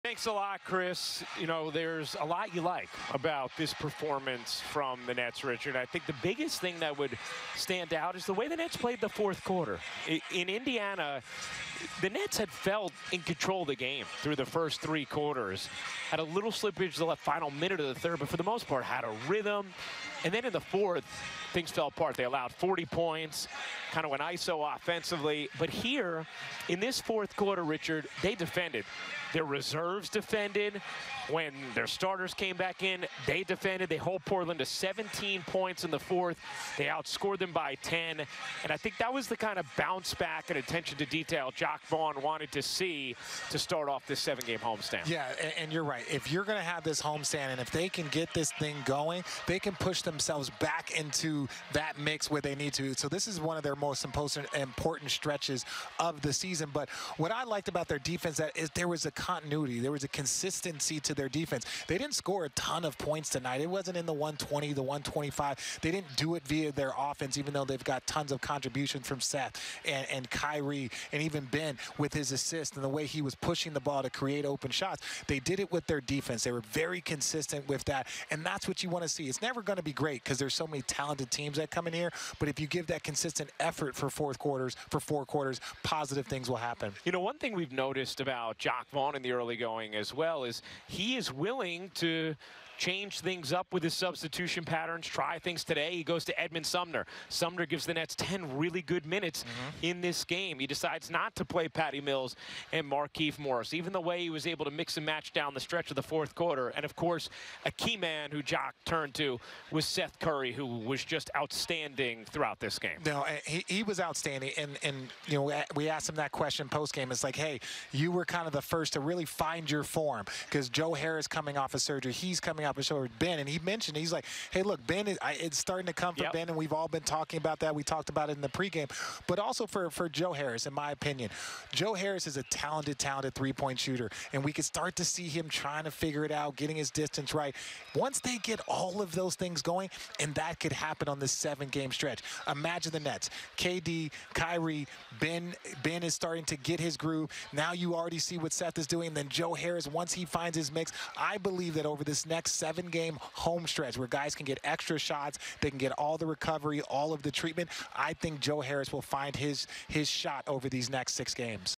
Ryan Ruocco and Richard Jefferson on the Nets' win
Ryan Ruocco and Richard Jefferson discuss the Nets’ win and look ahead to their next 6 games at home.